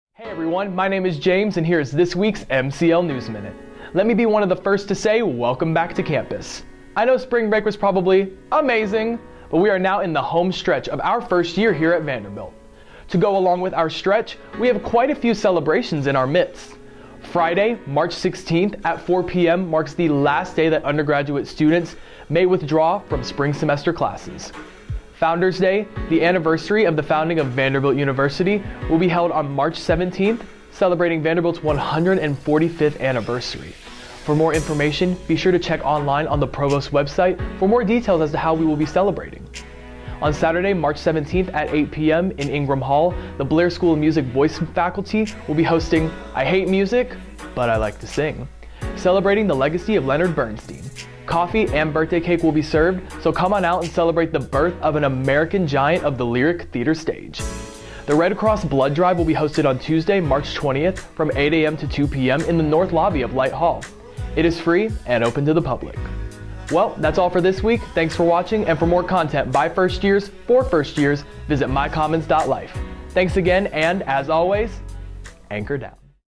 MyCommons Life News Minute: March 14, 2018
As we begin to wind down the academic year, James is here to welcome you all back from spring break as well as share some information with you about some celebrations within our midsts!